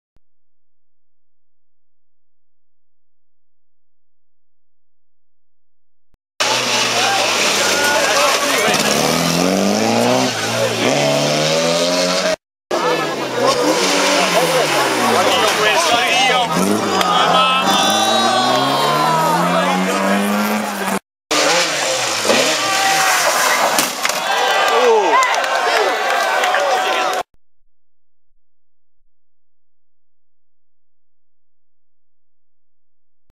legend 2010
innesto strerrato asfalto nella PS8 le ultime auto erano al buio totale...e la curva non si vedeva poi così tanto bene...e neanche il palo. :-))